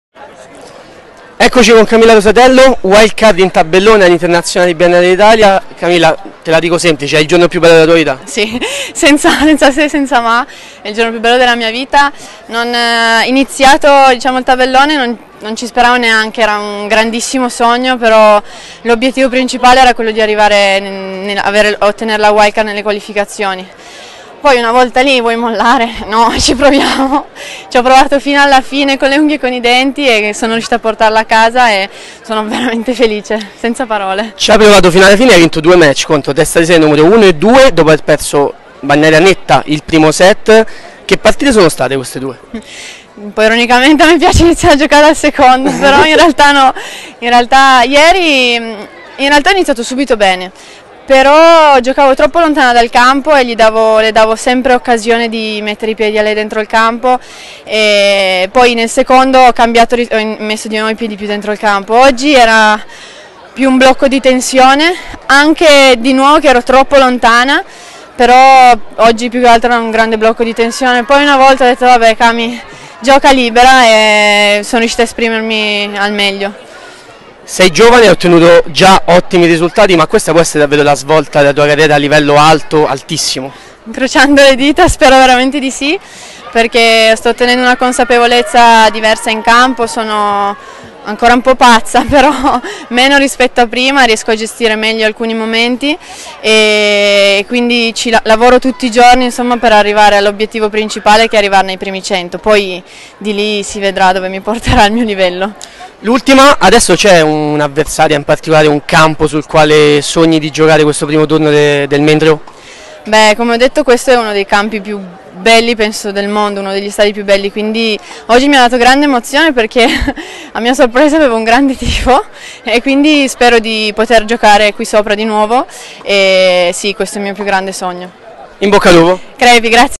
Prequalificazioni Internazionali BNL d'Italia, Rosatello: "Il giorno più bello della mia vita"
Le parole della tennista classe 1995 dopo la wild card conquistata in finale su Martina Trevisan: "Quando ho iniziato il tabellone non ci speravo neanche"